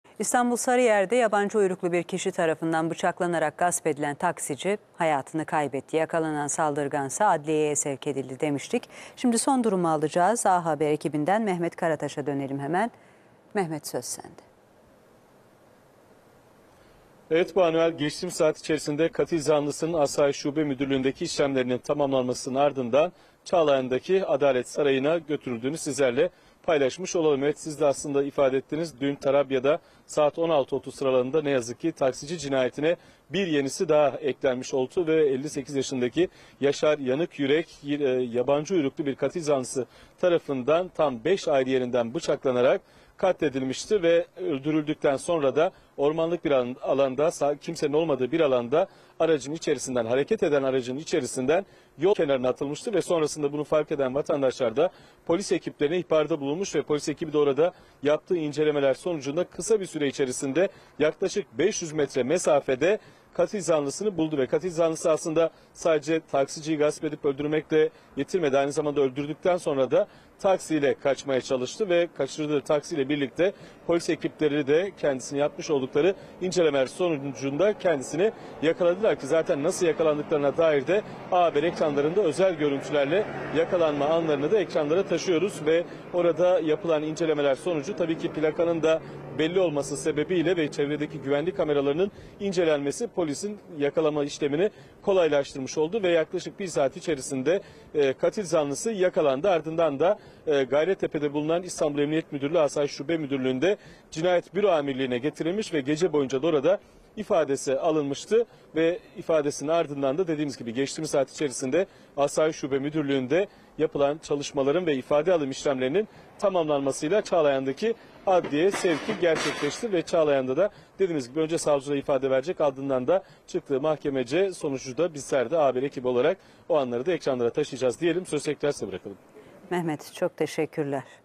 Sarıyer'de taksi şoförünü öldürüp otomobilden attılar! Katilin yakalanma anları A Haber'de
Sarıyer’de korkunç bir olay yaşandı. İddiaya göre, taksici Yaşar Yanıkyürek, aracı gasp edildikten sonra bıçaklanarak yol kenarına atıldı. Olay yerinde hayatını kaybeden Yanıkyürek'in cenazesi Adli Tıp Kurumu'na kaldırıldı. İstanbul Valiliği Yanıkyürek'i bıçaklayarak öldüren kişinin yakalandığını duyurdu. Katil tutuklanma talebiyle nöbetçi hakimliğe sevk edildi. Taksici katilinin yakalanma anına ise A Haber ulaştı.